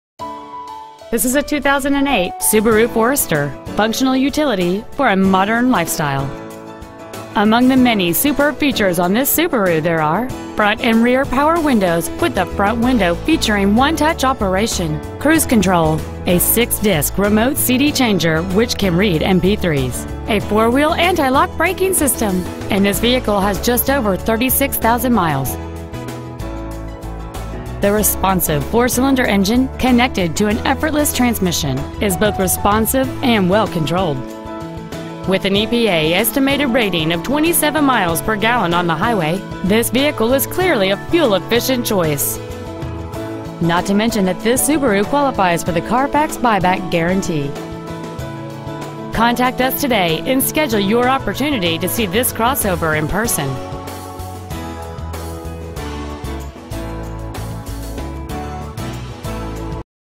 Pre-Owned 2008 Subaru Forester Denver CO 80221
201 W. 70th Ave.

 Year: 2008
 Make: Subaru
 Model: Forester
 Engine: 2.5L H-4cyl

 Exterior: Red
 Miles: 36,172
 Interior: Beige